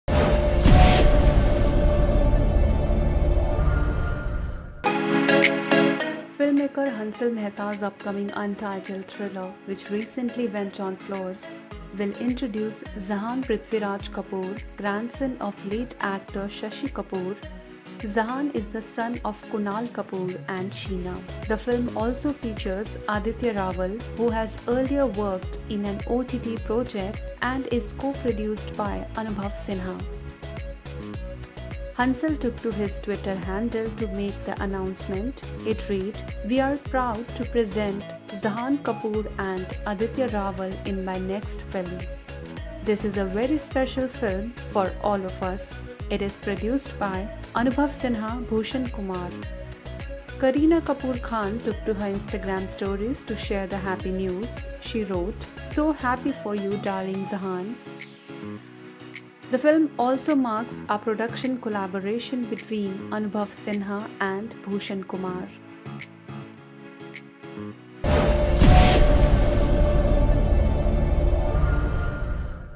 Shashi Kapoor's grandson Zahan Kapoor to make his acting debut in Hansal Mehta's next | WeForNews
Filmmaker Hansal Mehta's upcoming untitled thriller, which recently went on floors, will introduce Zahan Prithviraj Kapoor, grandson of late actor Shashi Kapoor. Zahan is the son of Kunal Kapoor and Sheena.